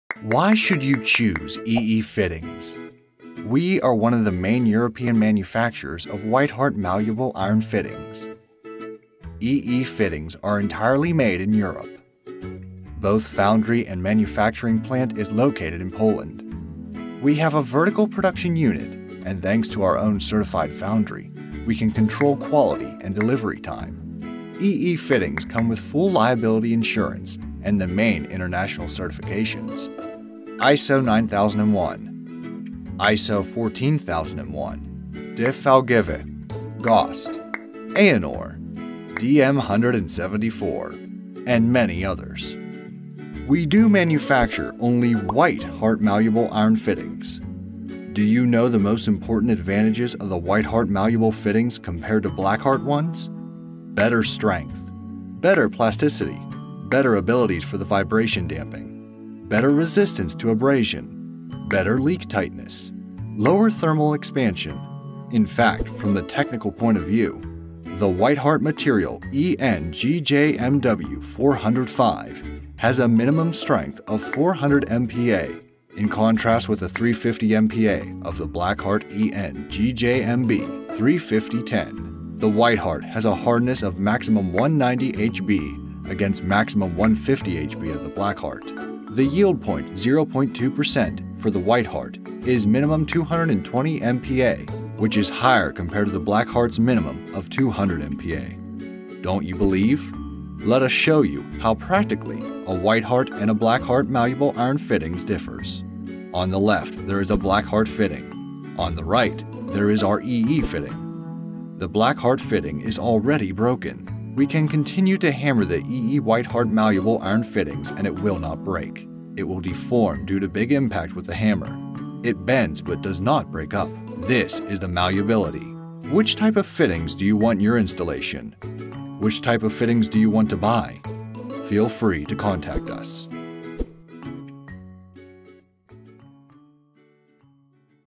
Difference Between White Heart and Black Heart Malleable Iron Fittings
Discover the difference between White Heart and Black Heart malleable iron fittings

Graphic Motion Animation Made By YourVideoscribe.com